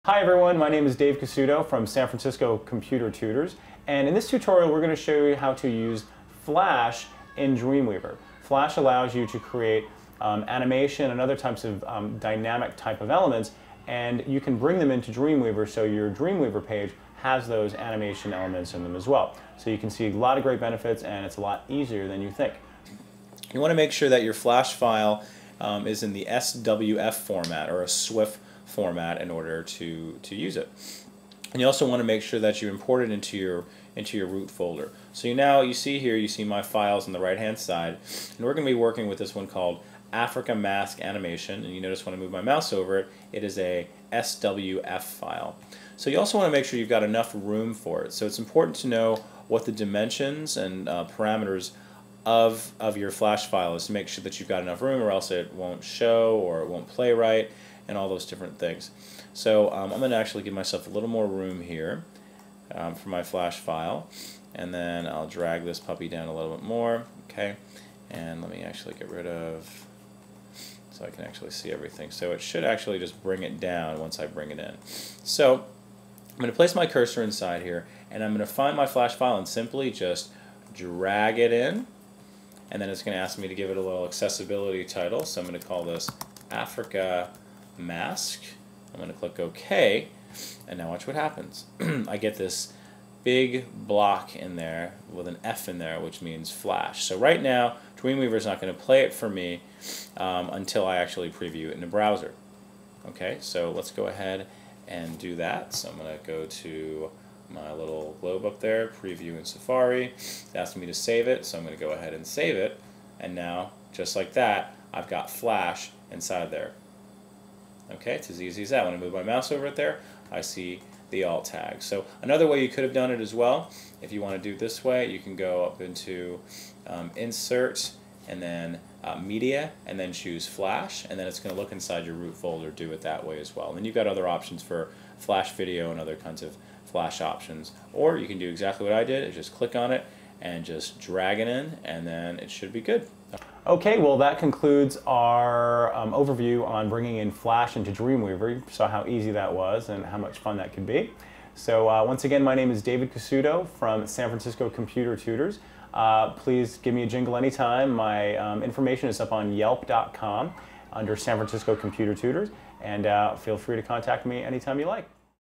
Dreamweaver Tutorial : How to Insert a Flash or Shockwave Movie Into a Web Page With Dreamweaver
With Dreamweaver, a Flash or Shockwave movie can be inserted into a Web page by placing the cursor where the video should go, finding the Flash file, which should be in Swift Format, and dragging the file into the desired area. Easily include video on a Web page with instructions from a certified Dreamweaver tutor in this free video on Web design.

Expert: Dave Casuto
Bio: Dave Casuto is a certified Dreamweaver tutor working in San Francisco, Calif.